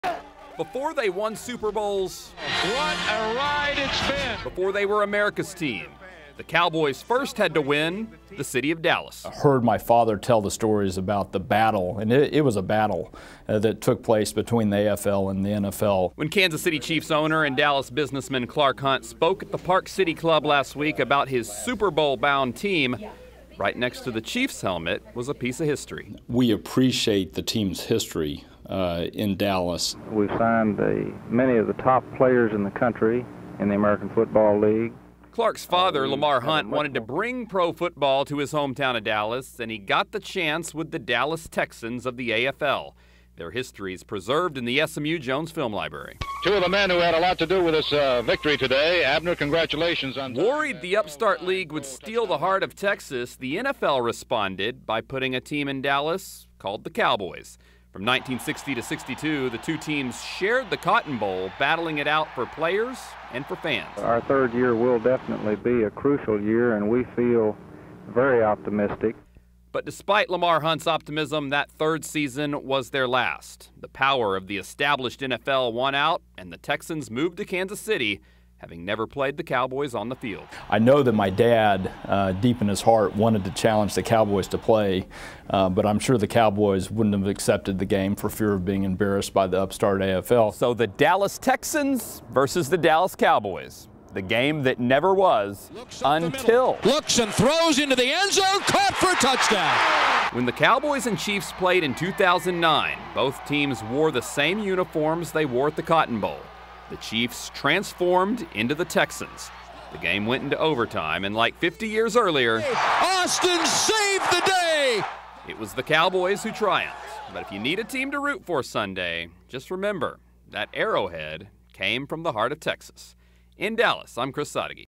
REWIND: Kansas City Chiefs Super Bowl run revives memory of Dallas Texans
From 1960-62, the Dallas Texans and Dallas Cowboys battled for the city in "The Game that Never Was."